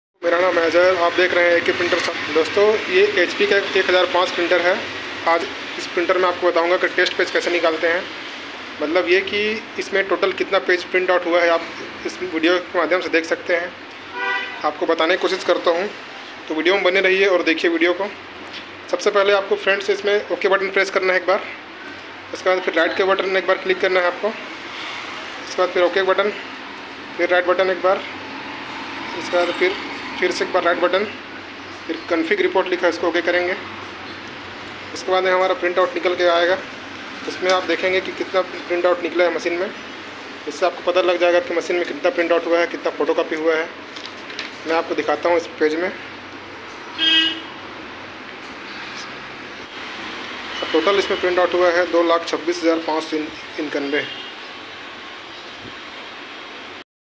How to Test page print ll hp 1005 Total print kaise nikale
your queries -
hp 1005 printer test page kaise nikale
hp 1005 printer test page
hp 1005 kitna chala hai kaise pata kare
hp 1005 print out

hello dosto aaj ki is video me maine aapko hp 1005 printer me test kaise niklate hai aur aapke printer me total kitna page nikla hai vo kaise pata kre
to dekhiye aur sikhiye